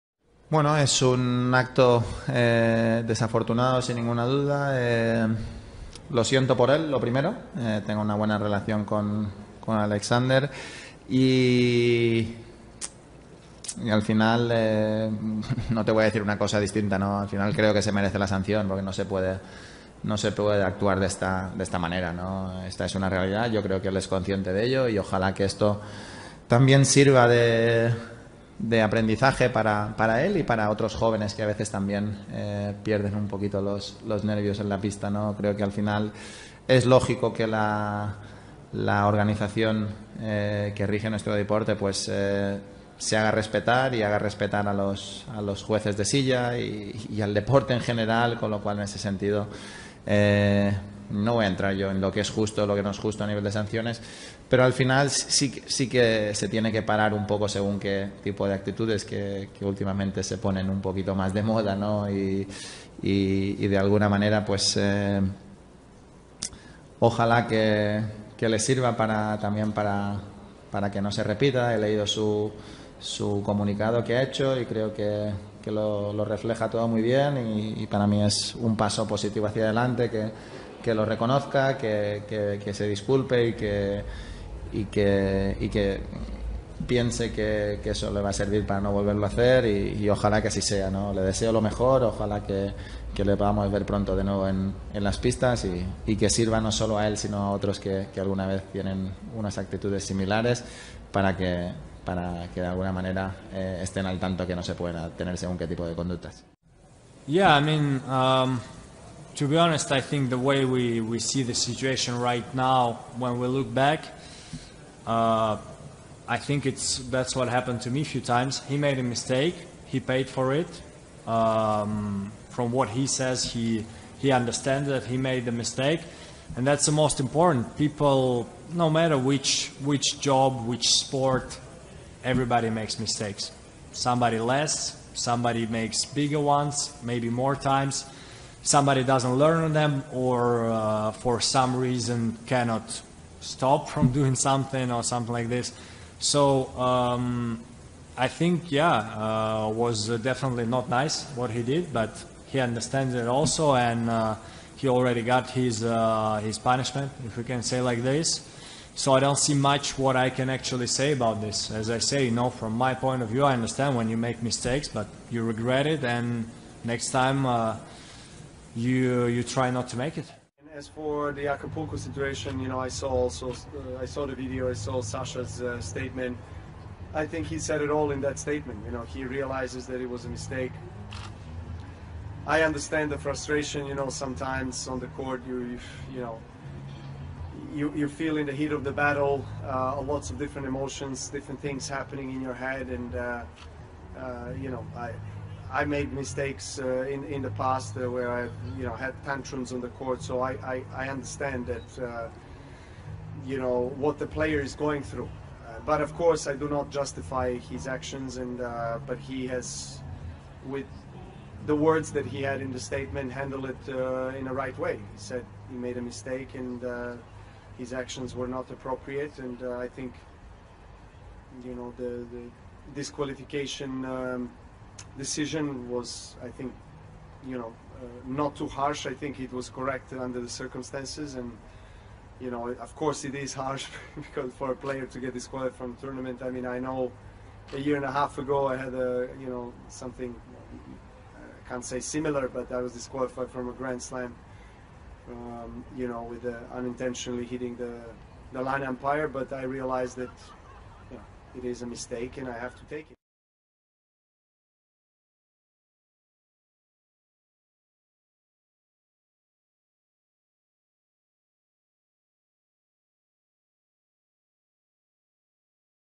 NADAL y DJOKOVIC OPINAN sobre ZVEREZ - conferencia de prensa - acapulco 2022 - umpire - zverev angry
Opinion de Djokovic y Nadal sobre actuacion de Zverev.
Zverev hits chair of chair umpire and got disqualified